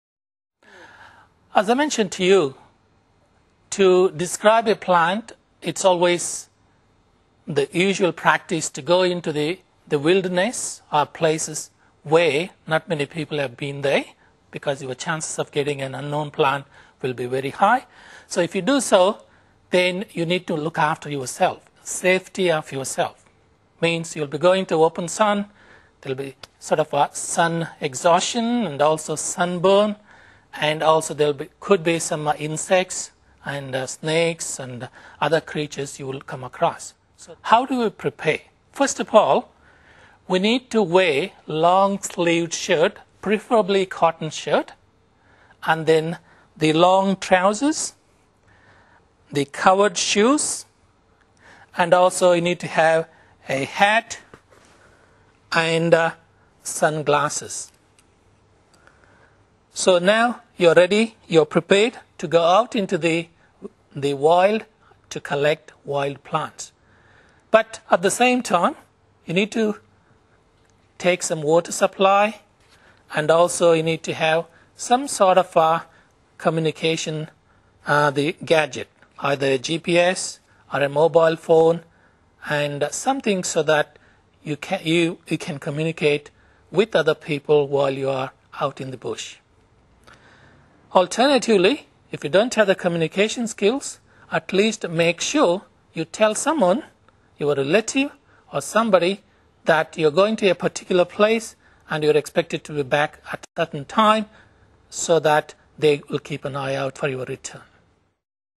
Herbarium Techniques Part 3 (getting ready)
This is part 3 of 10 parts and it explains how you should prepare yourself to go out for field collection